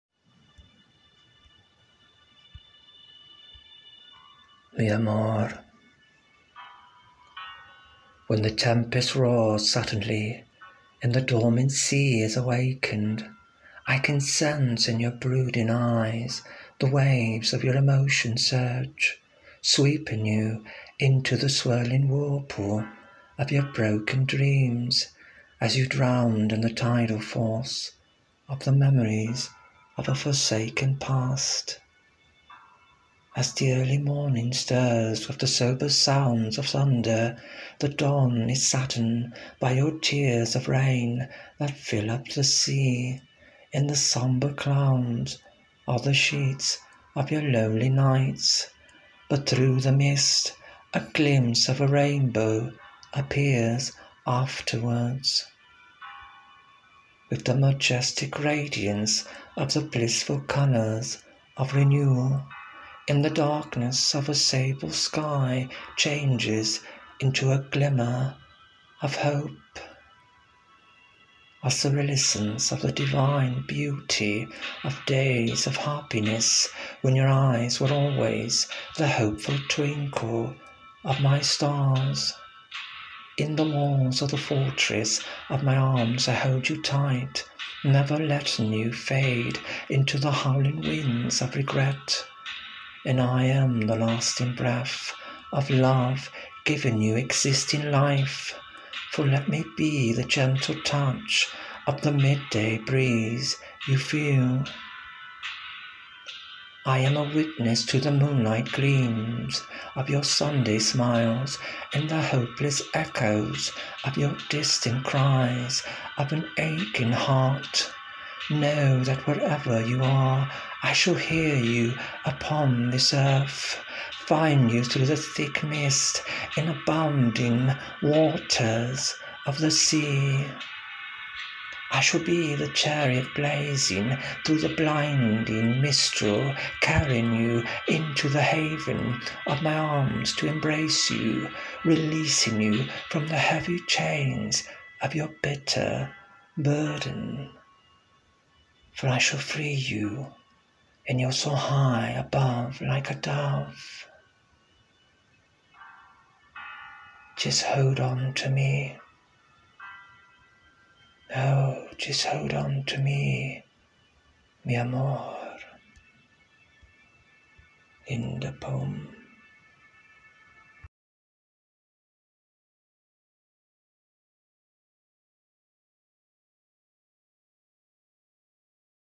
Hold On To Me
LNP poet Poetic Picasso reads his poem Hold On To Me for the show Hearts of LNP.